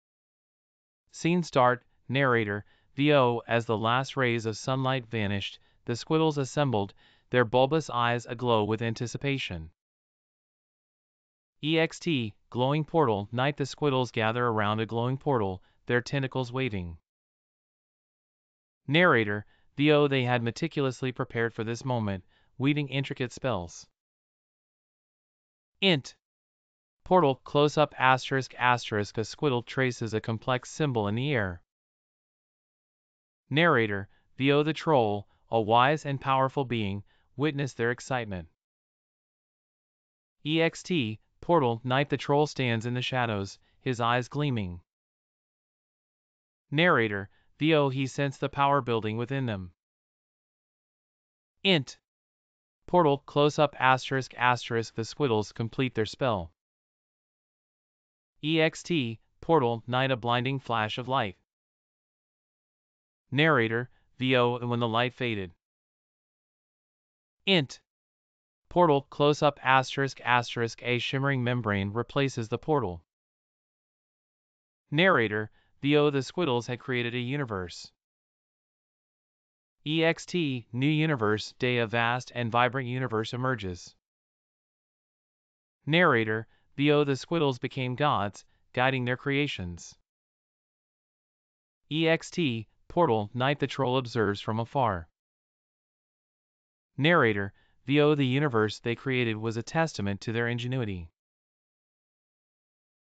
Squiddles: Creators of Universes
Witness the squiddles' magical creation of a vibrant universe, becoming gods to guide their flourishing civilizations, observed by a wise troll in awe.